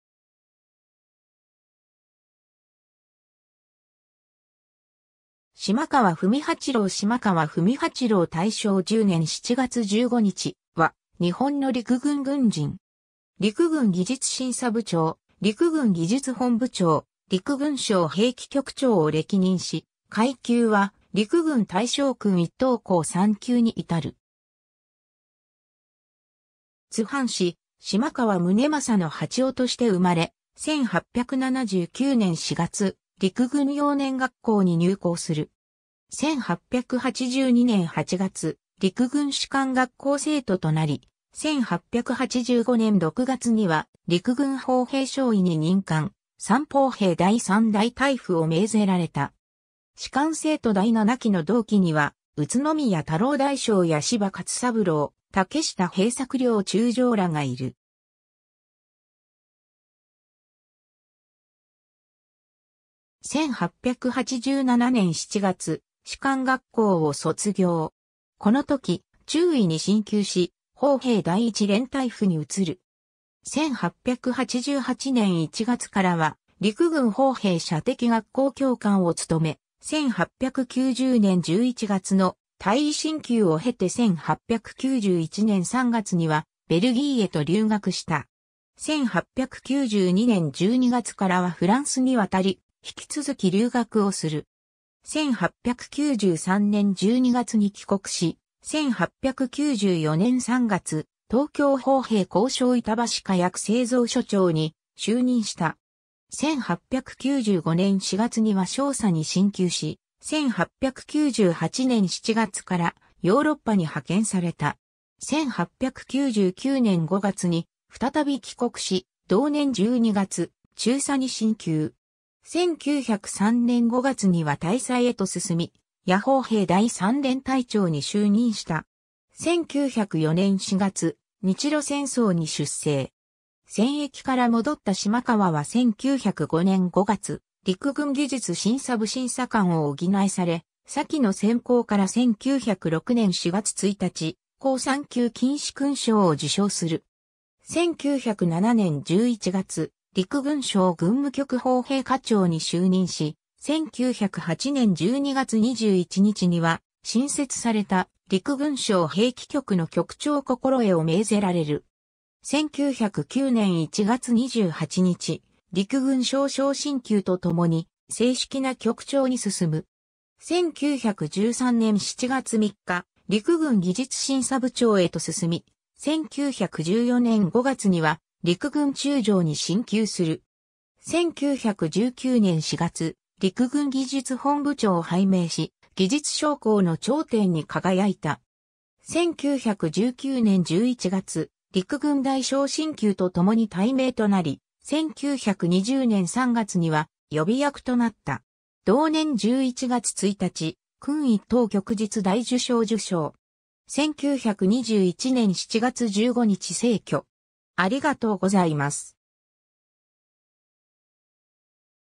島川文八郎
島川文八郎 島川 文八郎（しまかわ ぶんはちろう、元治元年3月10日（1864年4月15日） - 大正10年（1921年）7月15日）は、日本の陸軍軍人。
陸軍技術審査部長・陸軍技術本部長・陸軍省兵器局長を歴任し、階級は陸軍大将勲一等功三級に至る。
 津藩士・島川宗政の八男として生まれ、1879年（明治12年）4月、陸軍幼年学校に入校する。
1882年（明治15年）8月、陸軍士官学校生徒となり、1885年（明治18年）6月には陸軍砲兵少尉に任官・山砲兵第3大隊附を命ぜられた。
士官生徒第7期の同期には宇都宮太郎大将や柴勝三郎・竹下平作両中将らがいる。
 1887年（明治20年）7月、士官学校を卒業。
この時、中尉に進級し、砲兵第1連隊附に移る。
1888年（明治21年）1月からは陸軍砲兵射的学校教官を務め、1890年（明治23年）11月の大尉進級を経て1891年（明治24年）3月にはベルギーへと留学した。
1892年（明治25年）12月からはフランスに渡り、引き続き留学をする。
 1893年（明治26年）12月に帰国し、1894年（明治27年）3月、東京砲兵工廠板橋火薬製造所長に就任した。
1895年（明治28年）4月には少佐に進級し、1898年（明治31年）7月からヨーロッパに派遣された。
1899年（明治32年）5月に再び帰国し、同年12月、中佐に進級。
1903年（明治36年）5月には大佐へと進み、野砲兵第3連隊長に就任した。
 1904年（明治37年）4月、日露戦争に出征。
戦役から戻った島川は1905年（明治38年）5月、陸軍技術審査部審査官を補され、先の戦功から1906年（明治39年）4月1日、功三級金鵄勲章を受章する。
1907年（明治40年）11月、陸軍省軍務局砲兵課長に就任し、1908年（明治41年）12月21日には新設された陸軍省兵器局の局長心得を命ぜられる。
 1909年（明治42年）1月28日、陸軍少将進級と共に正式な局長に進む。
1913年（大正2年）7月3日、陸軍技術審査部長へと進み、1914年（大正3年）5月には陸軍中将に進級する。
1919年（大正8年）4月、陸軍技術本部長を拝命し、技術将校の頂点に輝いた。
1919年（大正8年）11月、陸軍大将進級と共に待命となり、1920年（大正9年）3月には予備役となった。
同年11月1日、勲一等旭日大綬章受章。
1921年（大正10年）7月15日逝去。